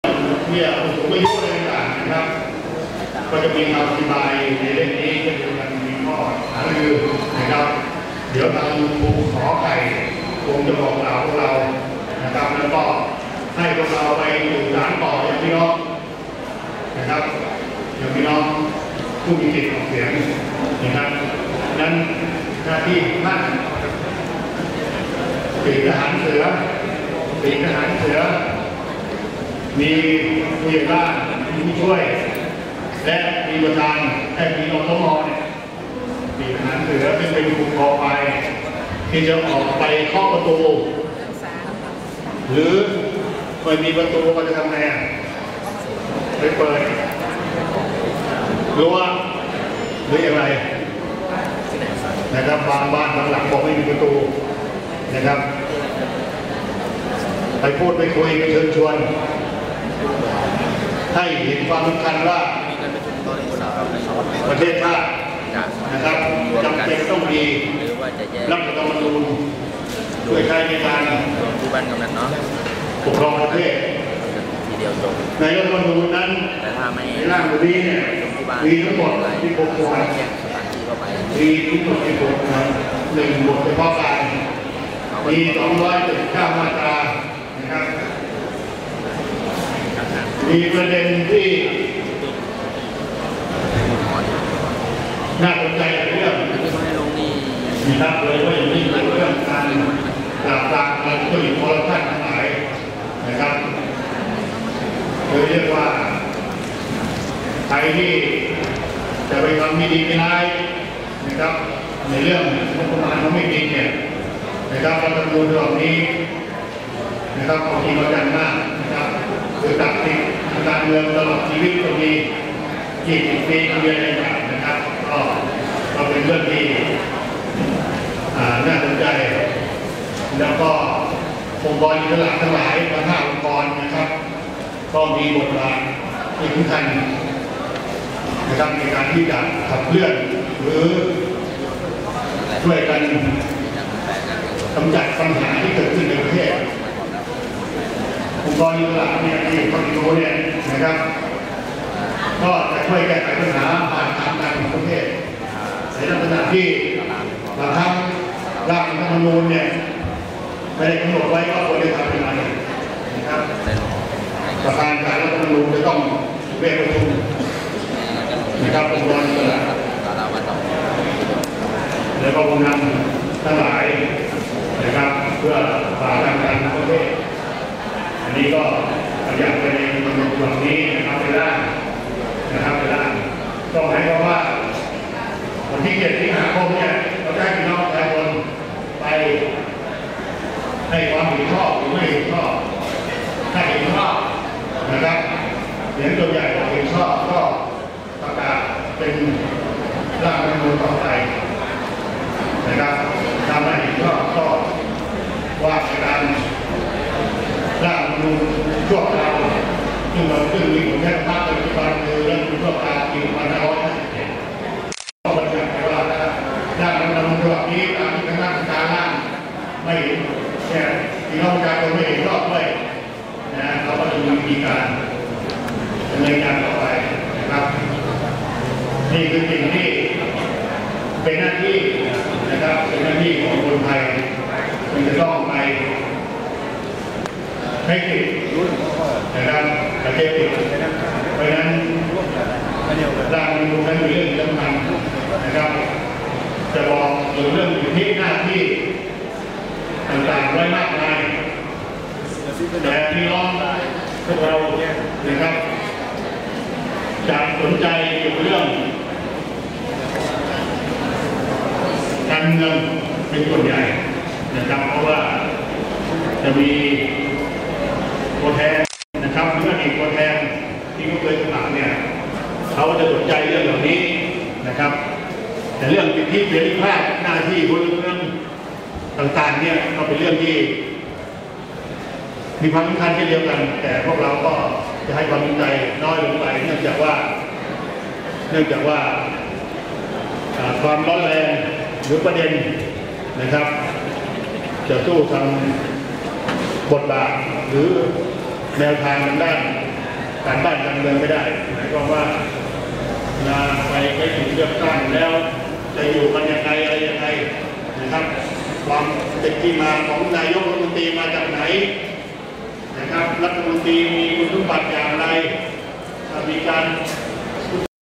1/3_อบรมวิทยากรครู ค. เผยแพร่ร่างรัฐธรรมนูญ จังหวัดบุรีรัมย์ 14 มิ.ย.2559
การอบรมอาสาสมัครเผยแพร่ประชาสัมพันธ์ร่างรัฐธรรมนูญ พ.ศ.๒๕๕๙ ระดับหมู่บ้านและชุมชน (วิทยากร ครู ค.) อำเภอเมืองบุรีรัมย์ รุ่นที่ ๑ จำนวน ๖๘๐  คน  วันที่ ๑๔ มิถุนายน ๒๕๕๙ ณ หอประชุมอำเภอเมืองบุรีรัมย์ โดยนายภูมิจิตร ศรีวงษ์ราช นายอำเภอเมืองบุรีรัมย์ เป็นประธานในพิธีเปิดการอบรม  บันทึกภาพกิจกรรมโดย นายสุเทพ  พันธุระ  ผช.สสอ.เมืองบุรีรัมย์